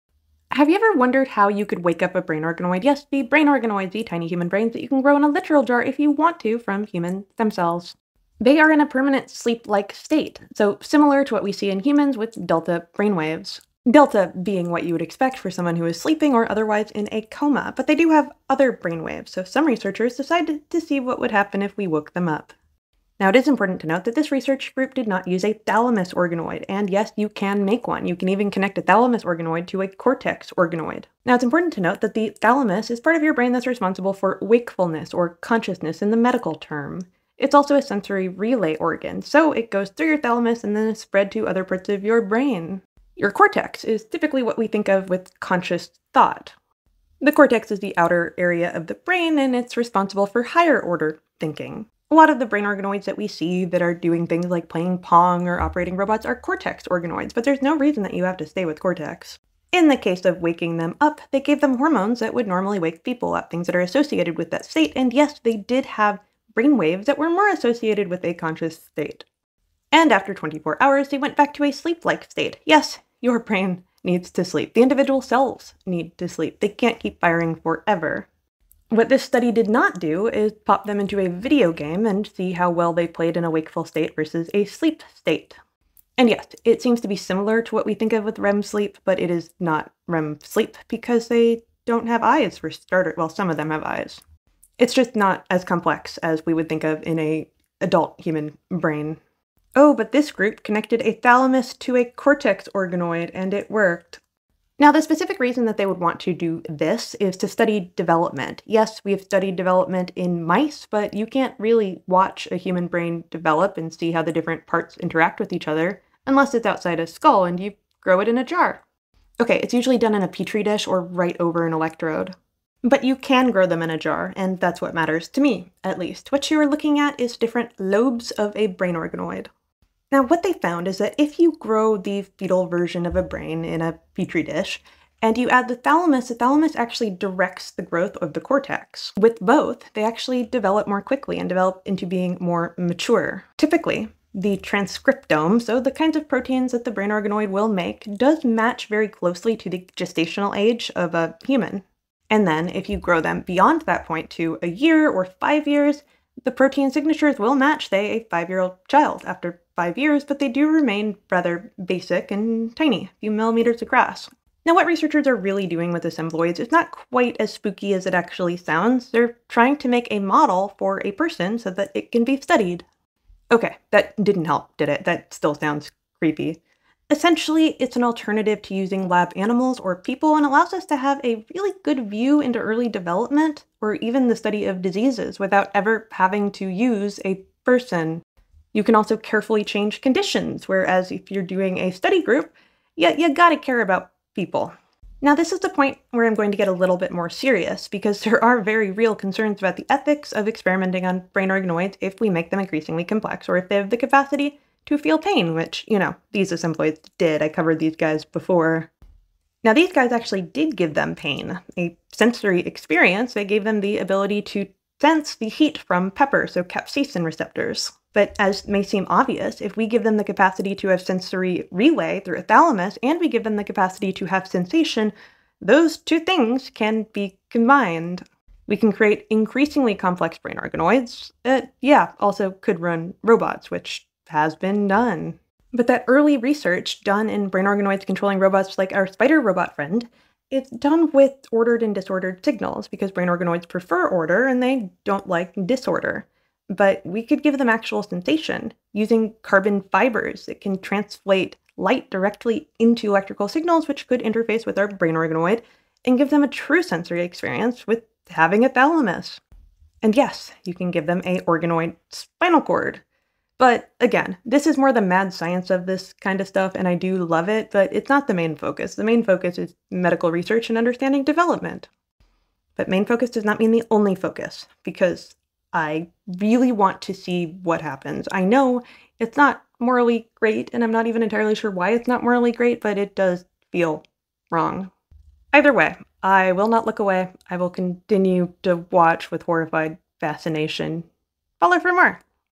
Lab Grown Mini Brains Can Be Woken Up From Sleep-Like States
Further Reading

Thalamus-cortex interactions drive cell type-specific
cortical
development in
human pluripotent
stem cell-derived

Key Electrophysiological, Molecular, and Metabolic
Signatures of Sleep and Wakefulness Revealed in Primary
Cortical Cultures

Complex Oscillatory Waves Emerging from Cortical Organoids Model Early Human Brain Network Development

Japanese scientists just built human brain circuits in the lab